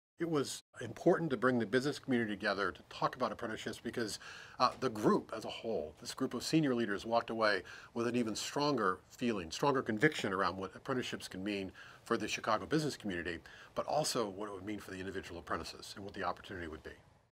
CEO Apprenticeship Roundtable- Greg Case, President and CEO, Aon
Why was it important to bring Chicago Business leaders together to talk about apprenticeships?

Hear from Greg Case, President & CEO, Aon.